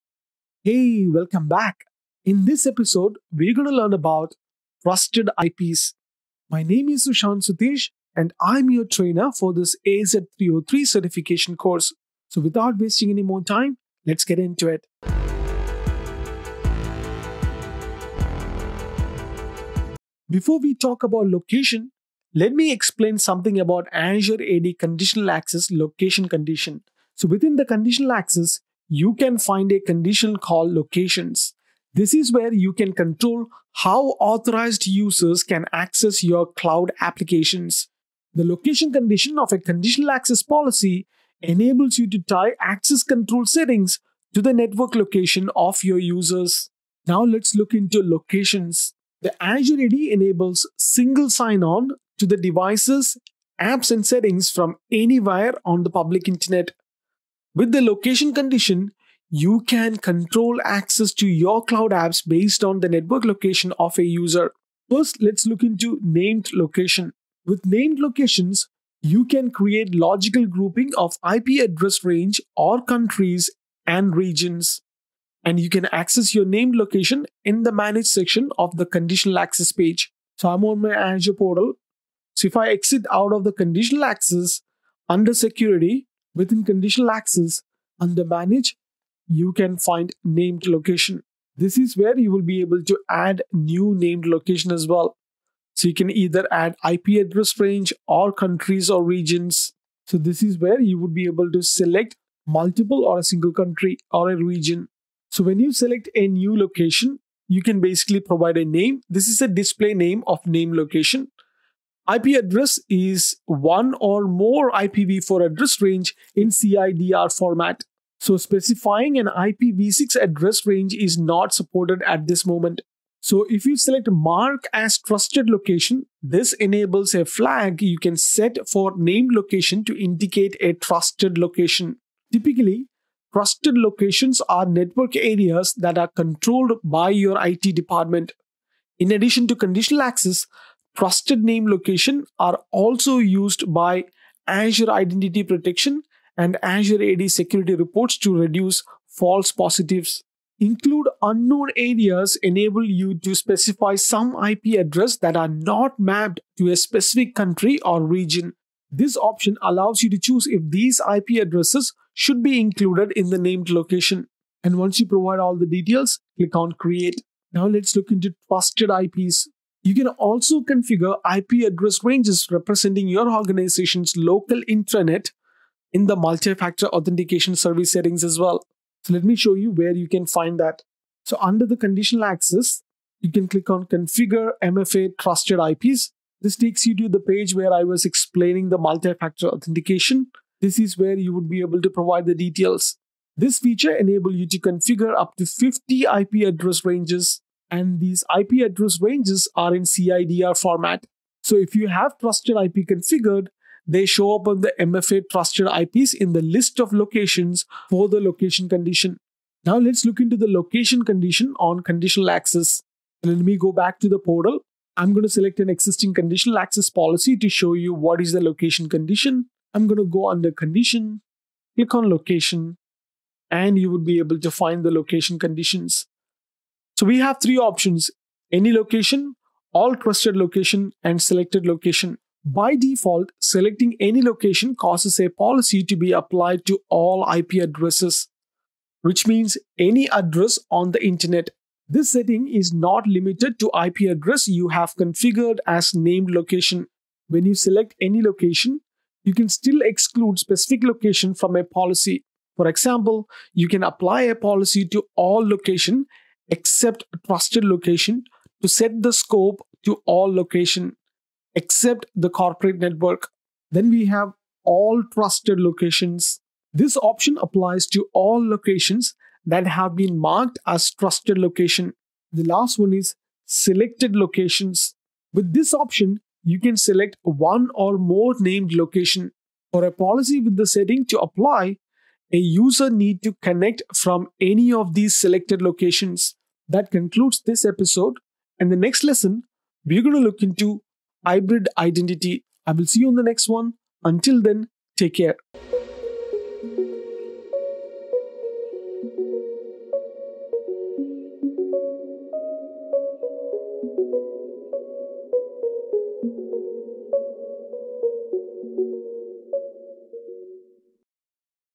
AZ-303 Exam EP 09: Configure Trusted IPs
⏰ Timestamps:

00:00​​ ⏩ Introduction
00:20​​ ⏩ Locations
01:05​​ ⏩ Named Locations
02:47​ ⏩ Trusted IPs
03:30 ⏩ Location Condition Configuration
04:46​​ ⏩ What's Next